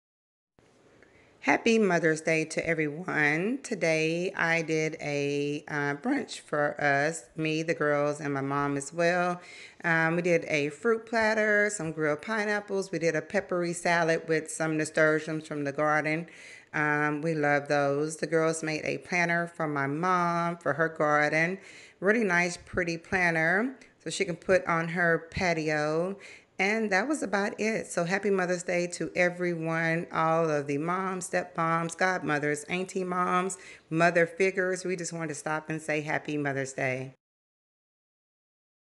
HAPPY MOTHER'S DAY!..🌺🌺🌺
Hope you all had a wonderful weekend! I just wanted to stop by and wish all Happy Mother's Day. I know I am a day late and I apologize. Today was my last final exam for the semester and I have been really busy. See you all soon.